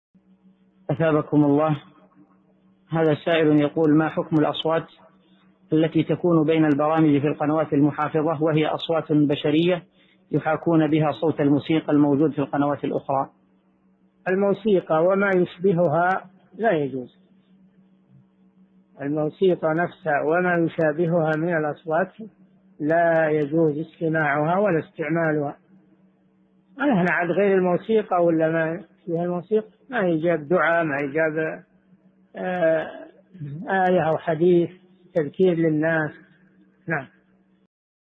[38 /186] ما حكم الأصوات التي تكون بين البرامج في القنوات المحافظة وهي أصوات... | الشيخ صالح الفوزان
[38 /186] ما حكم الأصوات التي تكون بين البرامج في القنوات المحافظة وهي أصوات بشرية يحاكون بها صوت الموسيقى الموجود في القنوات الأخرى؟
الجواب: الموسيقى وما يشبهها لا يجوز، الموسيقى نفسها وما يشبهها من الأصوات لا يجوز استماعها ولا استعمالها، ما هناك غير الموسيقى وإلا ما يشبه الموسيقى، ما يجاء بدعاء، ما يجاء بآية أو حديث تذكير للناس؟
السيرة الذاتية :
للاشتراك في قناة الشيخ صالح الفوزان

للاستفسار والإقتراحات والدعم
رئيس الفريق
لمشروع كبار العلماء